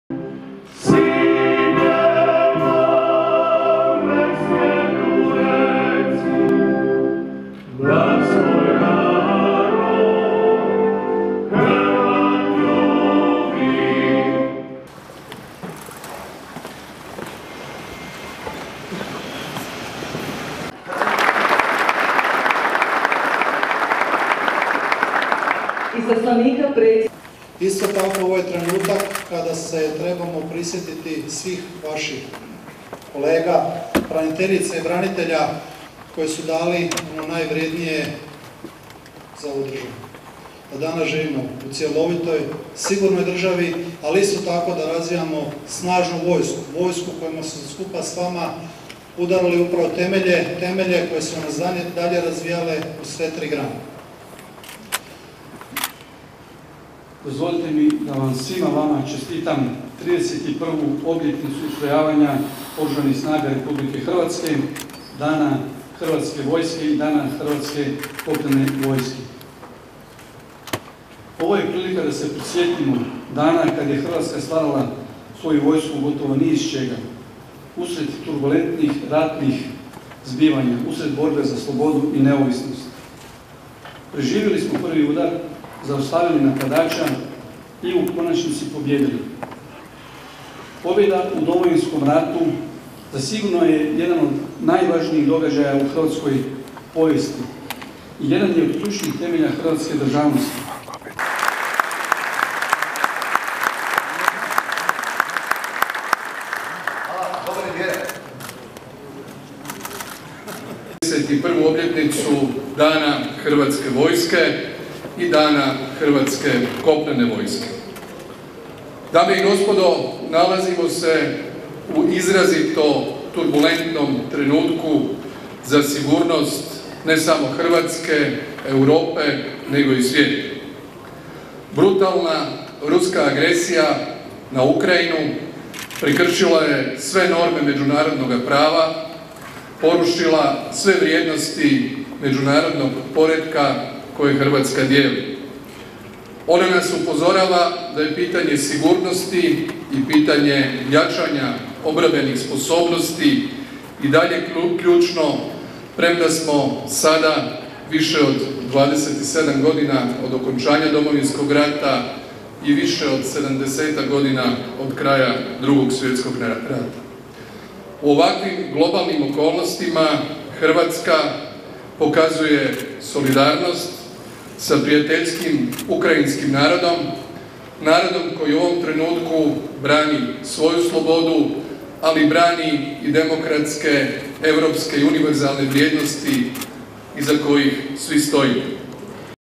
Svečana akademija u povodu obilježavanja Dana Hrvatske vojske
Zagreb, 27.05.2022 - Svečana akademija u povodu obilježavanja Dana Hrvatske vojske, Dana Hrvatske kopnene vojske i 31. obljetnice ustrojavanja Hrvatske vojske u Domu Hrvatske vojske 'Zvonimir' u Ministarstvu obrane.